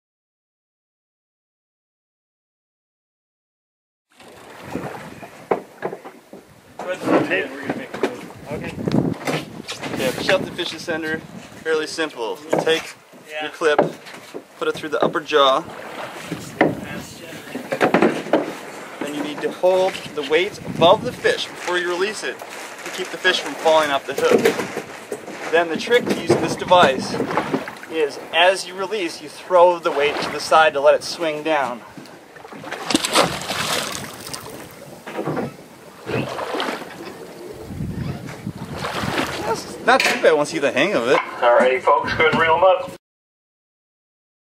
Shelton Fish Descender
Our marine biologists love rockfish descenders! Why? Because these handy little gizmos are an effective release tool anglers can use to help rebuild and extend bottom-fishing opportunities on the coast. Available at tackle shops up and down the coast, descenders help bottom fish get back to depths of 100 ft. or more … which keeps them from dying from “the bends” at the surface.